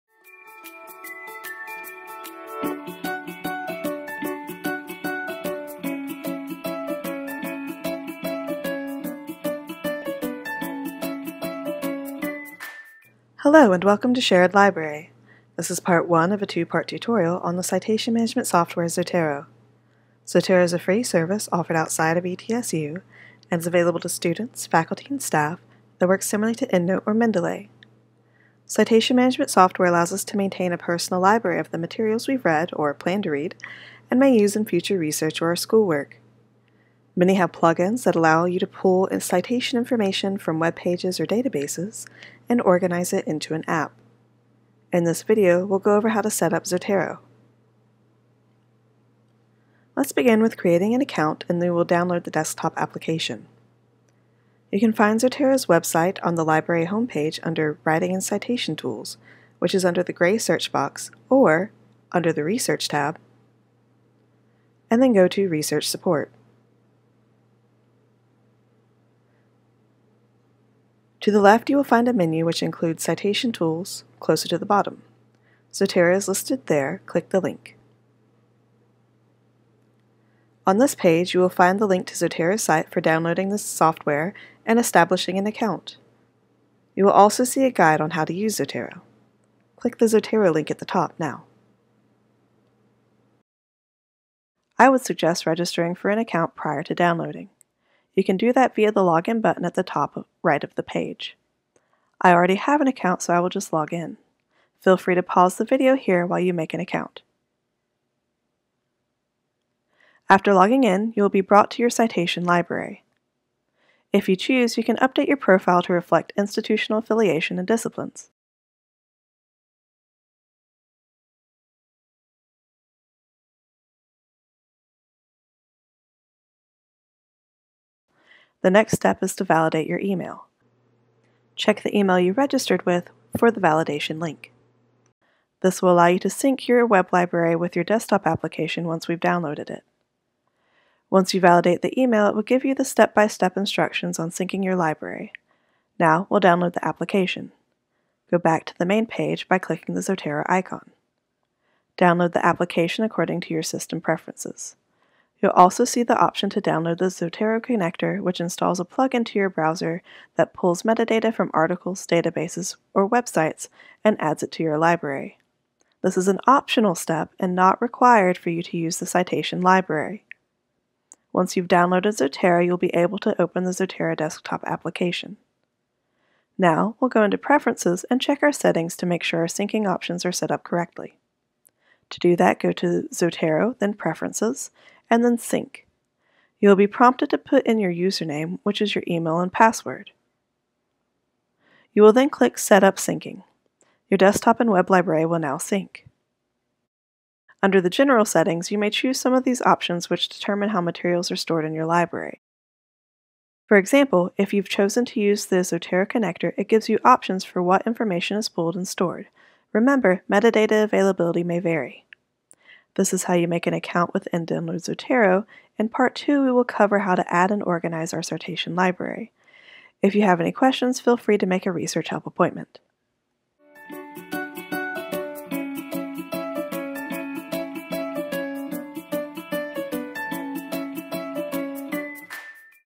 Sherrod Library Online Tutorials: Zotero Pt. 1
Learn how to use the reference management software Zotero in this online tutorial.

Was this tutorial helpful? Let us know